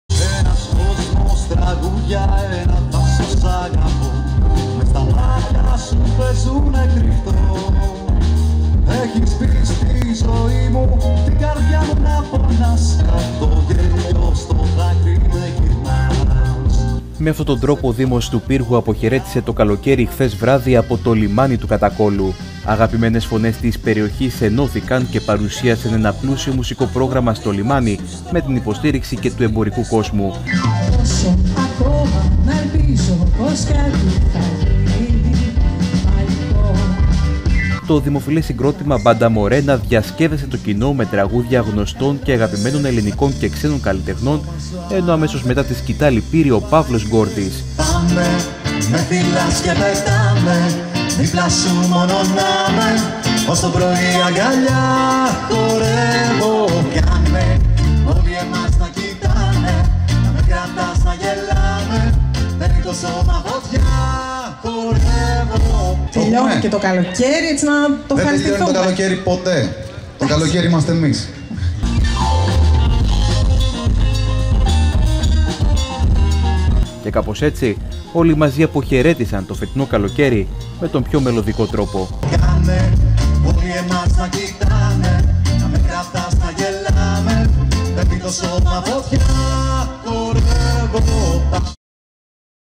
Ο Δήμος Πύργου αποχαιρέτησε το καλοκαίρι με συναυλία στο Κατάκολο
Με την συναυλία των BANDA MORENA στο Κατάκολο αποχαιρέτησε ο Δήμος Πύργου το φετινό καλοκαίρι, κλείνοντας ουσιαστικά με αυτόν τον τρόπο και το πλούσιο πολιτιστικό του πρόγραμμα.